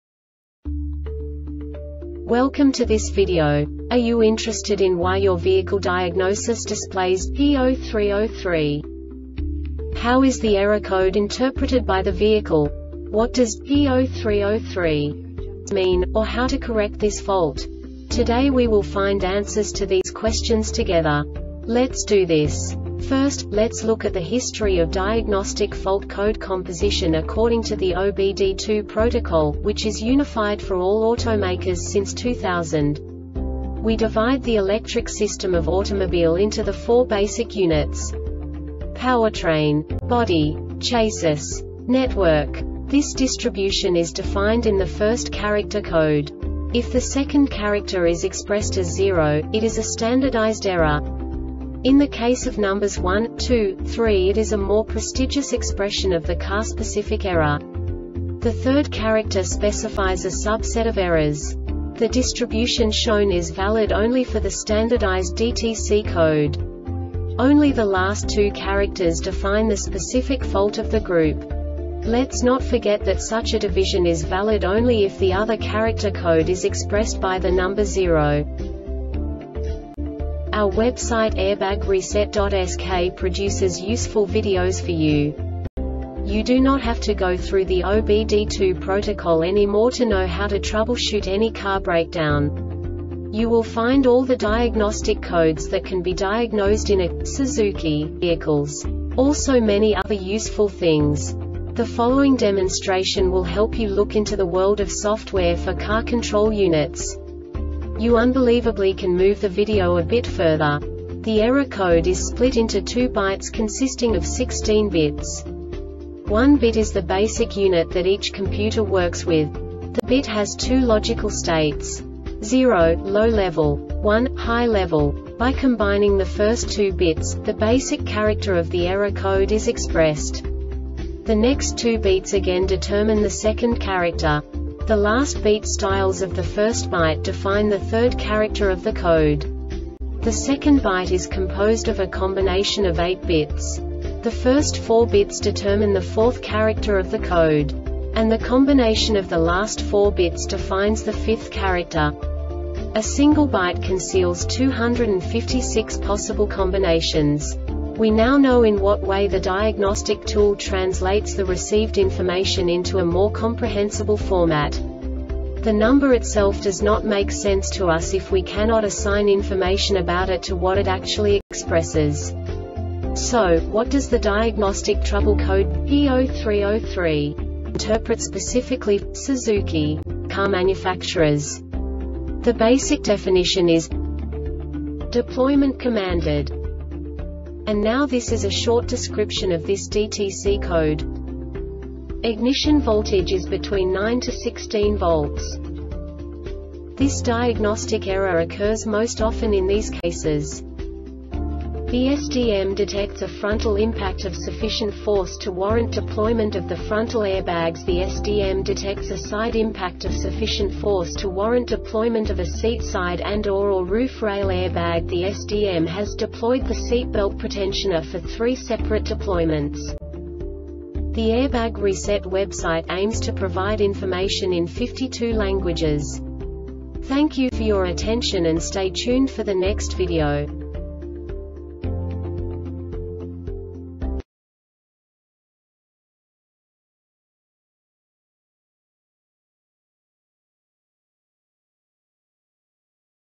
DTC Suzuki P0303 Short Explanation
Contents:
0:21 Basic DTC analysis according to OBD2 protocol standard.
1:48 Insight into programming
2:58 A diagnostically understandable description of the Diagnostic Trouble Code
3:05 Basic error definition

Make:
Suzuki
Code:
P0303
Definition:
Deployment Commanded
Description:
Ignition voltage is between 9-16 volts.
Cause:
The SDM detects a frontal impact of sufficient force to warrant deployment of the frontal air bags.The SDM detects a side impact of sufficient force to warrant deployment of a seat side and or or roof rail air bag.The SDM has deployed the seat belt pretensioner for 3 separate deployments.